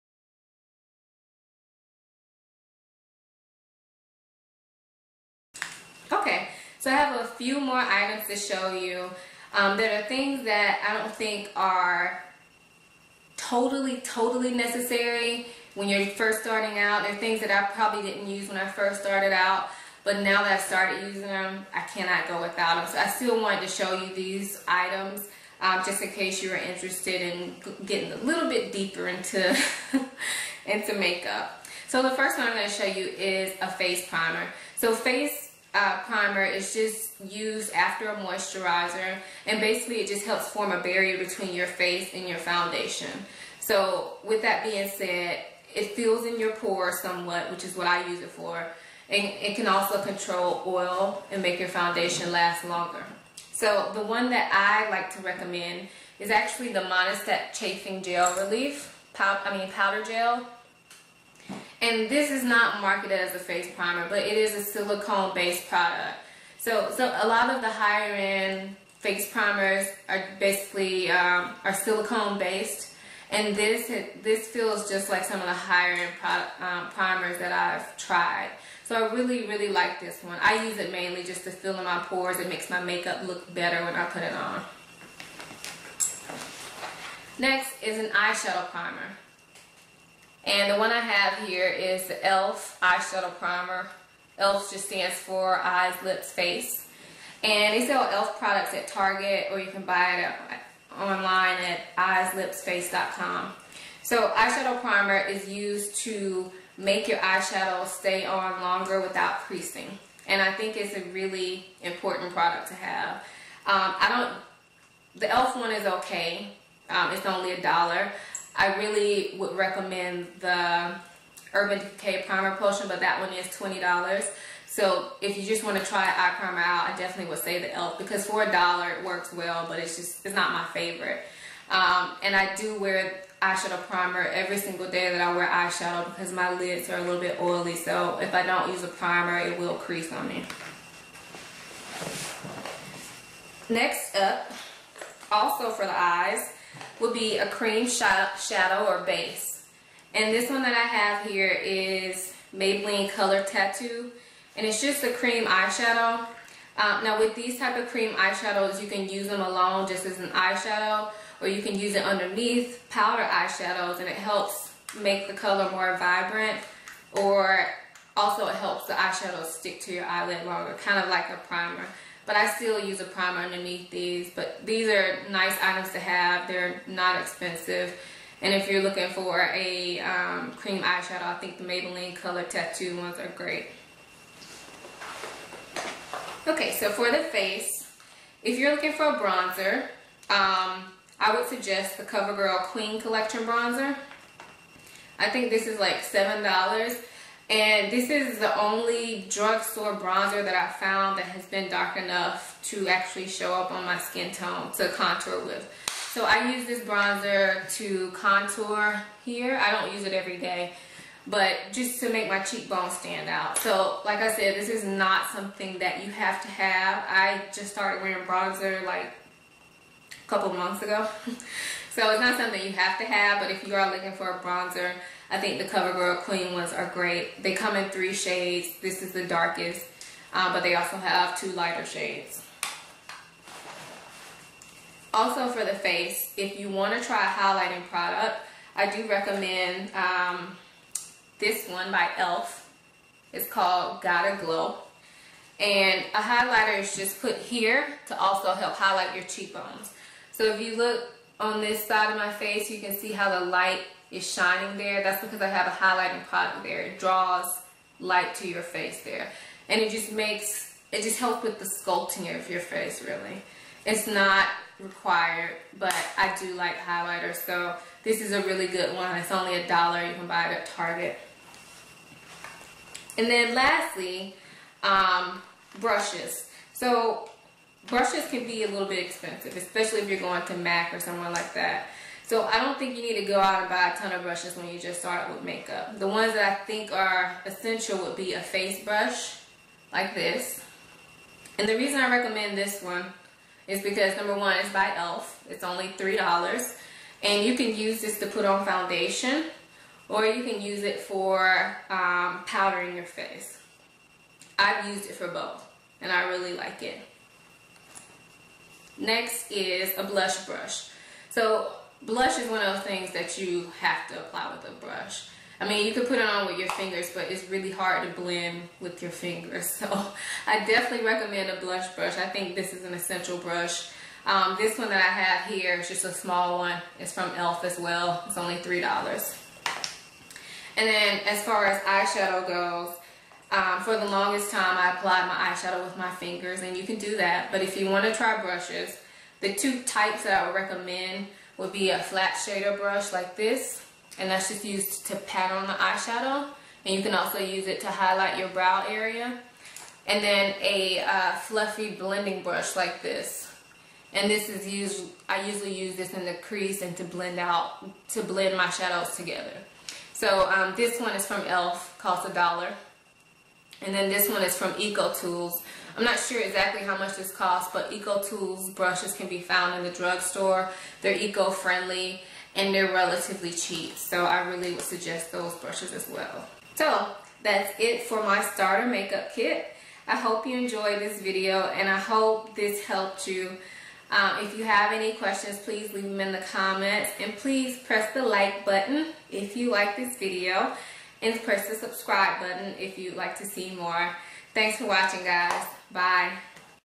Affordable Makeup Starter Kit Part 2
Products Mentioned:
Loreal True Match Lumi in C6 Soft Sable
Loreal True Match in N7 Natural Tan
Revlon Photoready Concealer in Deep
Wet n Wild Color Icon Blusher in Heather Silk
Wet n Wild Color Icon Eyeshadow Palette in Comfort Zone
NYX 10 Color Eyeshadow Palette in Bohemian Rhapsody
Milani Liquif'eye Mettalic Eyeliner in Black
Rimmel Exagerrate Eye Definer in Noir
Maybelline Eye Studio Gel Eyeliner in Blackest Black
Maybelline The Falsies Mascara in Very Black
Maybelline The Mega Plush Mascara in Very Black
Revlon Super Lustrous Lipstick Matte in Stormy Pink
NYX Round Lipstick in Echo
Loreal Colour Riche Caresse Lipstick in Sunset Angora
Monistat Chafing Relief Powder-Gel
ELF Eyelid Primer
Maybelline Color Tattoo in Bad to the Bronze
Covergirl Queen Collection Bronzer in Ebony Bronze
ELF Blush in Gotta Glow (Highlighter)
ELF Studio Powder Brush
ELF Studio Blush Brush
Ecotools Eyeshadow Brush
ELF Professional Blending Eye Brush